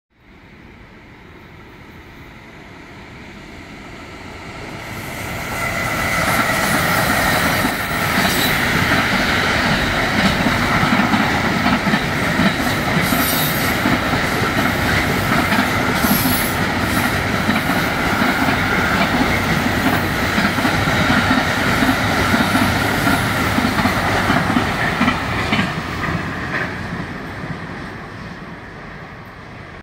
Vectron MIR e DB in doppia trazione con treno merci deviato dal Brennero 😀
Un treno merci deviato dal Brennero sulla linea Venezia - Udine con in testa una doppia trazione di Vectron, una in livrea Mercitalia e una in livrea rossa DB Cargo